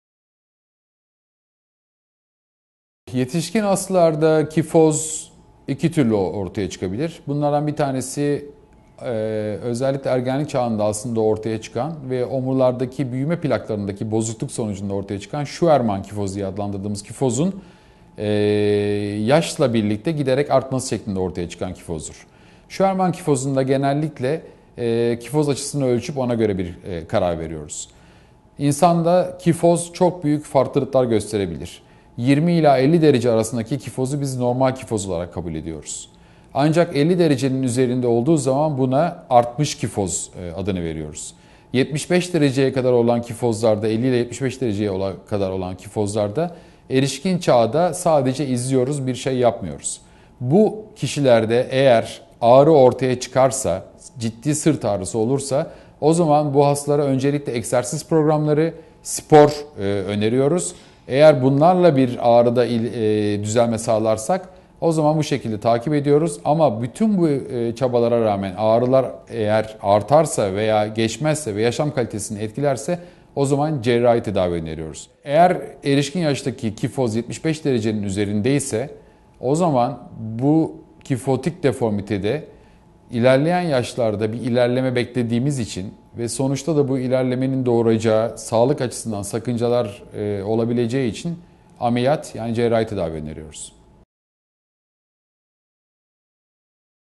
Yetişkinlerde Kifozun tedavisi nasıl olur?
Acıbadem Maslak Hastanesi, Ortopedi ve Travmatoloji Uzmanı Prof. Dr. Ahmet Alanay, Omurga Deformiteleri üzerine soruları yanıtlıyor;